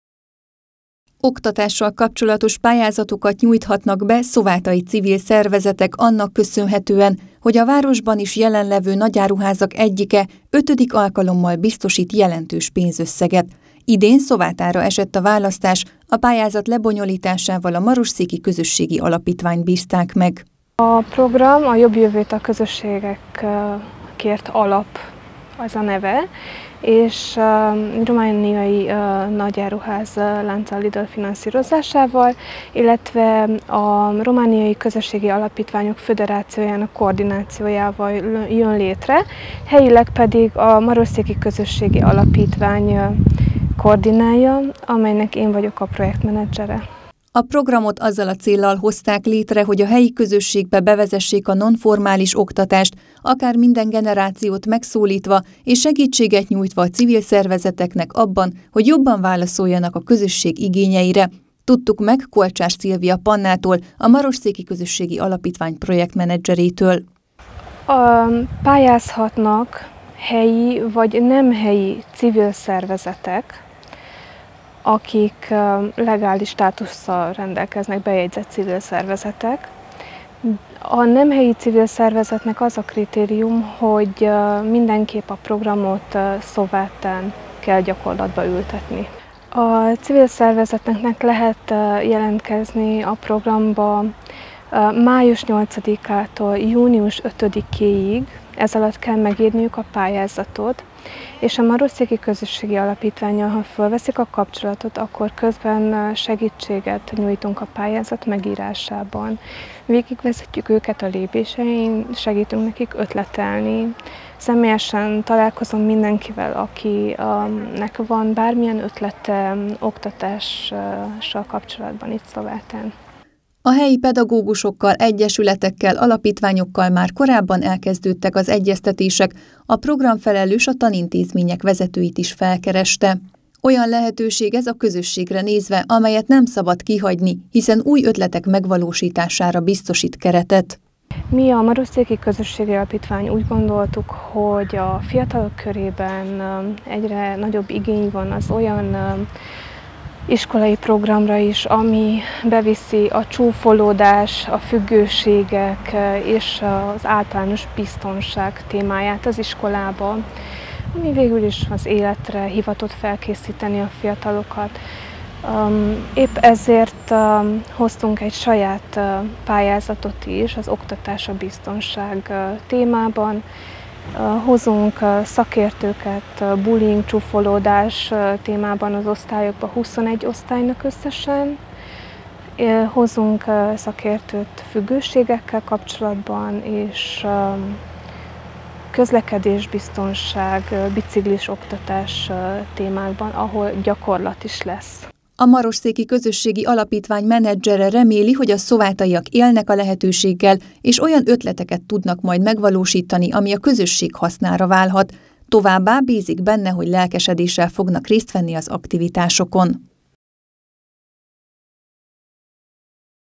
Szovátára esett a választás 🎯🏞️
Szovátai civil szervezetek oktatással kapcsolatsos pályázatokat nyújthatnak be, amelynek jelentős támogatást nyújt egy Szovátán is működő nagy áruház, a Marosszéki Közösségi Alapítvány hozzájárulásával.